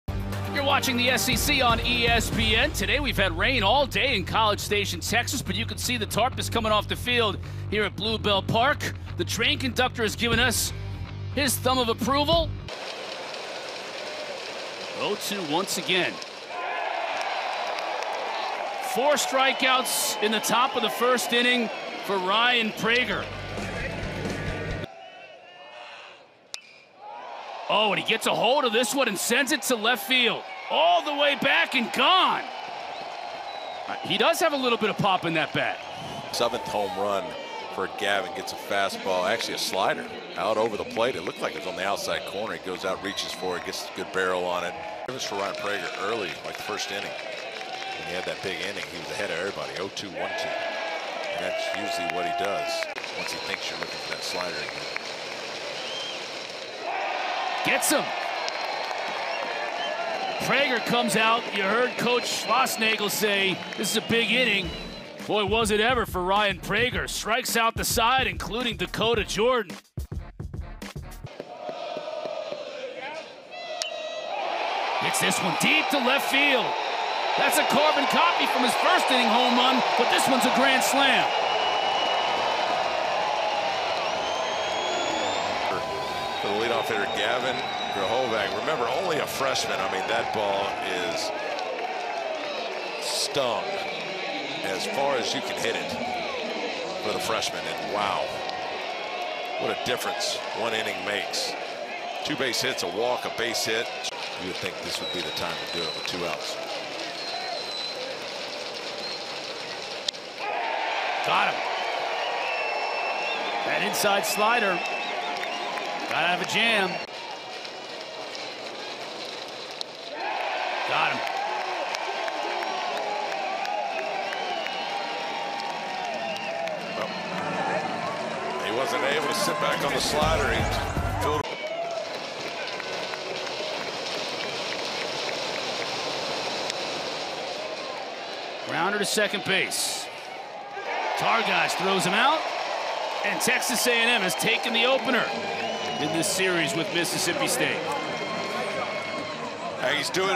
Highlights: A&M 6, Mississippi State 3
Watch Aggie baseball's highlights from the 6-3 win to open the series over Mississippi State at Blue Bell Park.